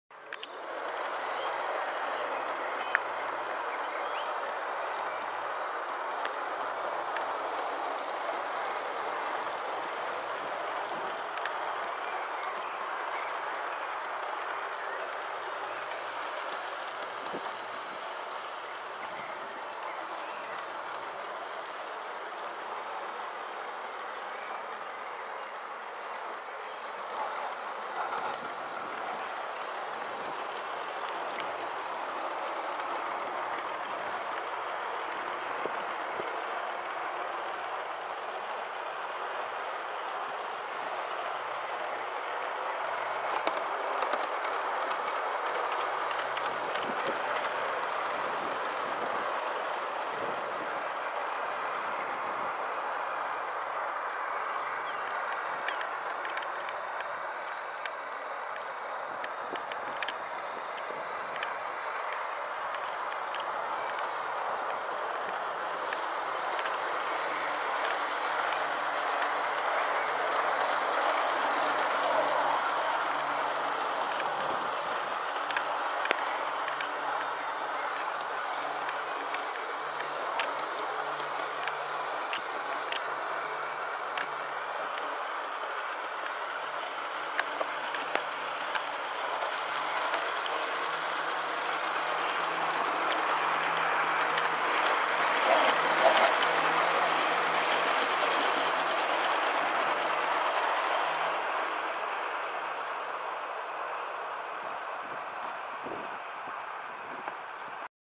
Class 66 G scale loco
Aristo-craft g scale loco, fitted with Electronize motor speed control and 2.4GHz cheap radio control from a R/C car. Batteries are 14 cells of rechargable NiMh. x12 sub c cells housed in underbelly fuel tank x2 cells inside body. All new ctl just hooked onto loco'soriginal 2pin battery connector. nice and simple and cheap.